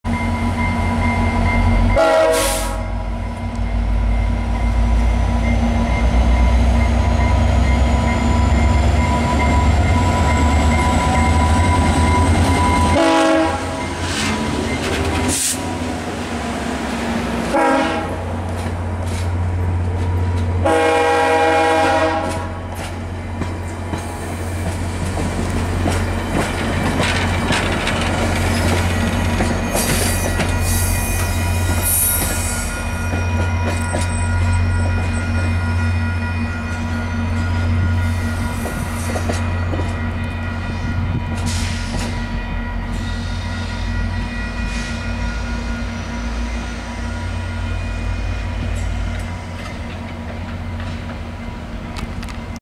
VIA Train #85 Departs Stratford
VIA Rail's train #85 departs Stratford Ontario for St. Mary's and London with F40PH-2 #6426, HEP-I coach #8124 and HEP-II VIA1 car #8005.